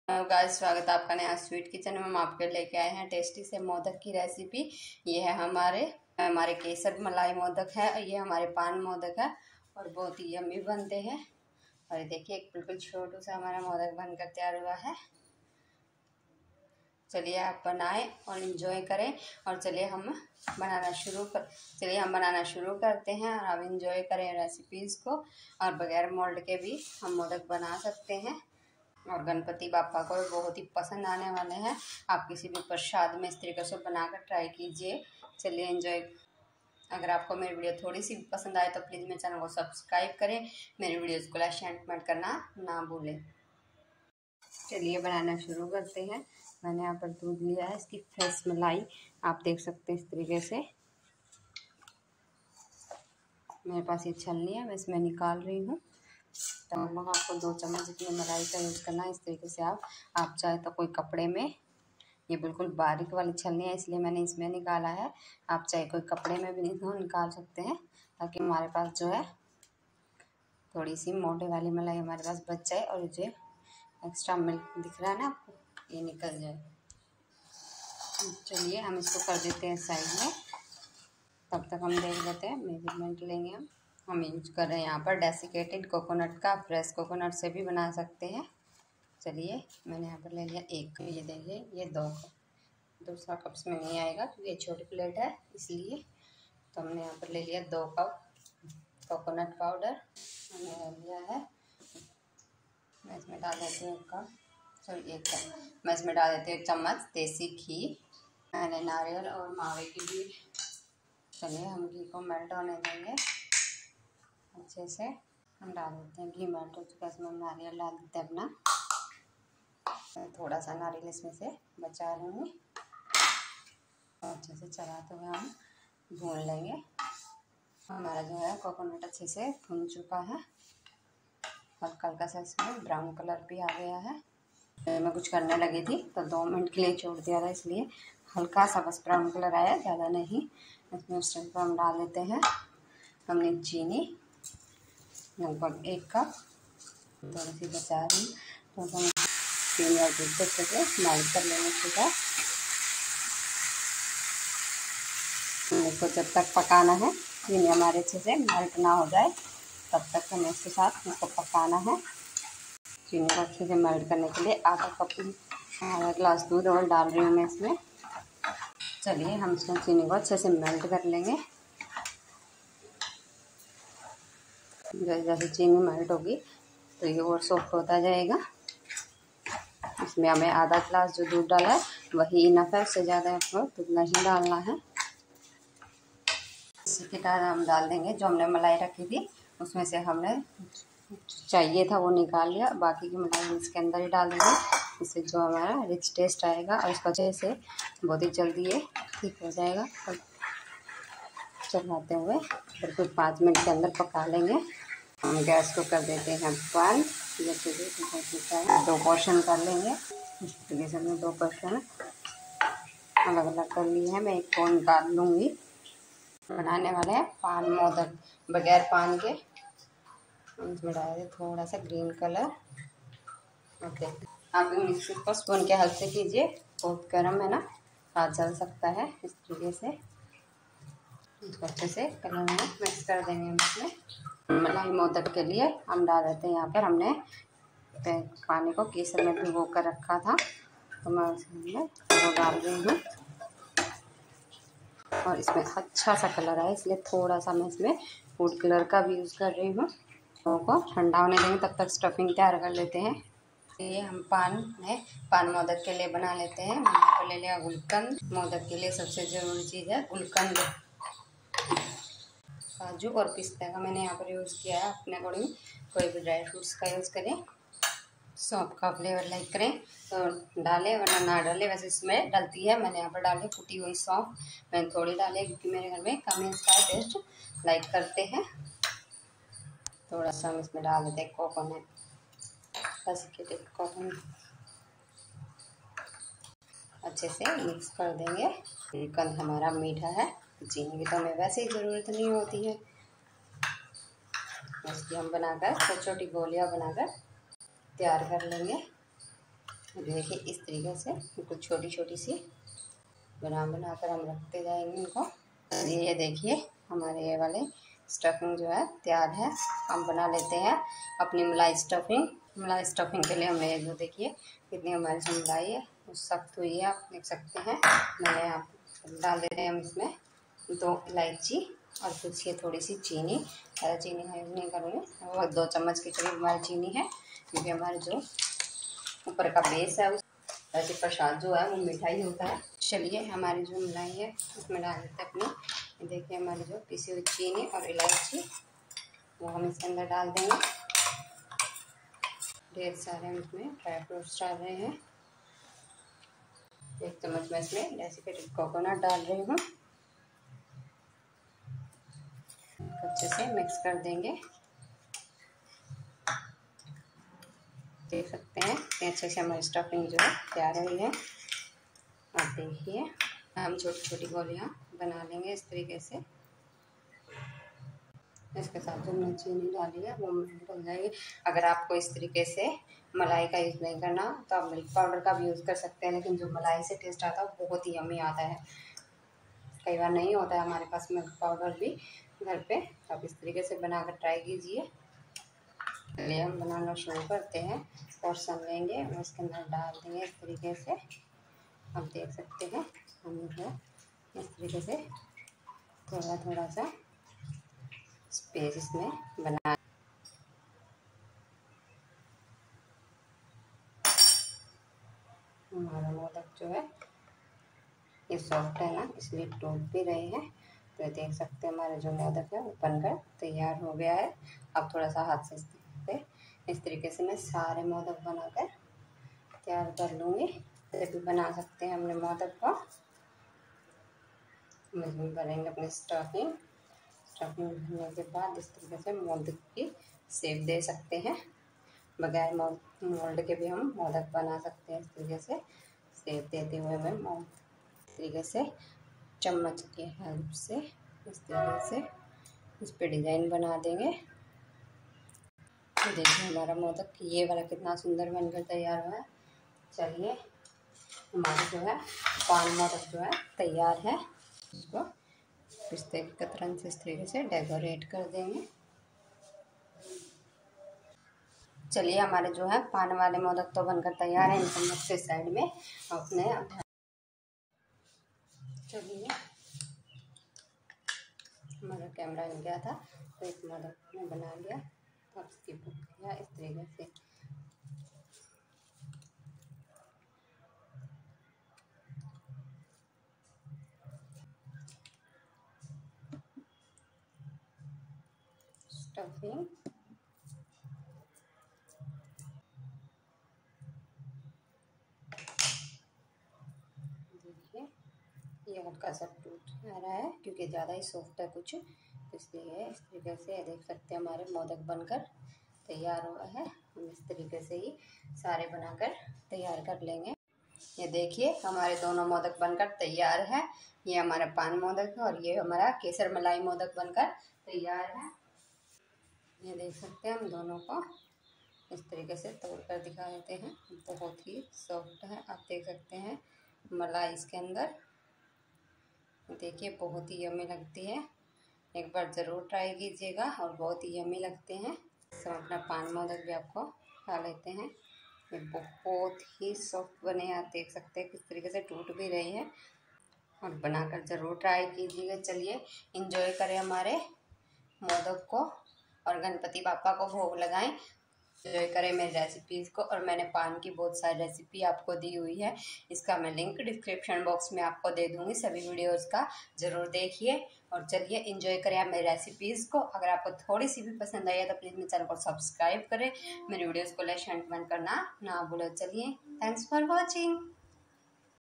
मलाई केसर,पान मोदक,2 तरीके से नए स्वाद मे प्रसाद बनाएँ ओर बाप्पा को भोग लगाये। Modak Recipe
Modak recipe
 kesar malayi modak, pan modak

ingredients
2cup coconut powder
1cup suger
2elichi
1tsp gulkand
2,3tsp malayi
Dry fruit accodding to you